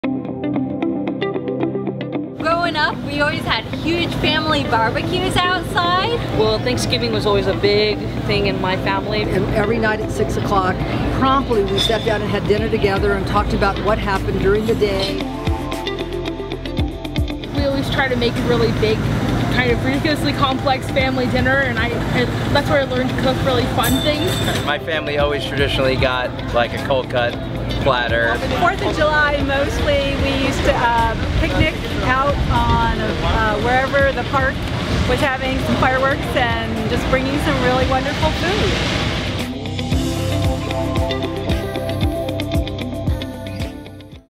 Dietz & Watson Family Meal Memories
While we were out on the road for our Four Generations of Flavor Tour, we asked some of our fans what their favorite family meal memories were. Here’s what they had to say, and share yours with us to have the chance to win a trip to Oktoberfest 2015 in Germany.